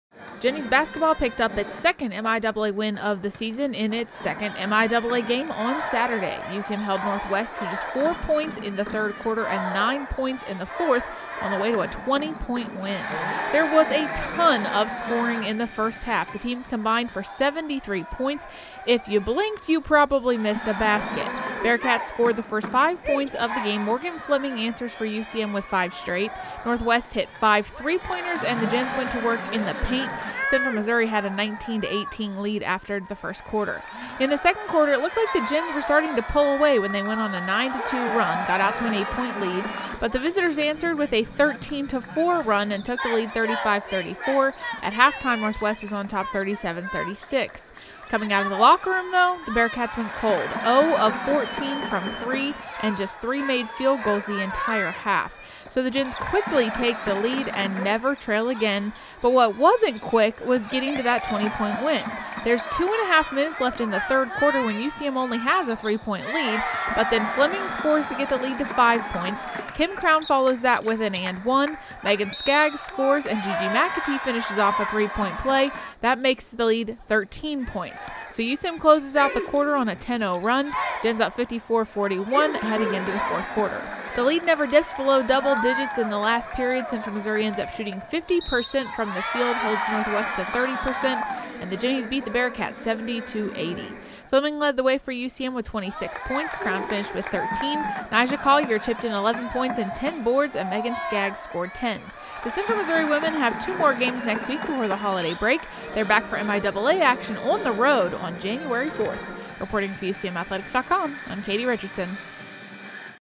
Jennies Basketball: UCM tops Northwest by 20
Central Missouri women's basketball beat Northwest 70-50 in the second MIAA game of the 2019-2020 season.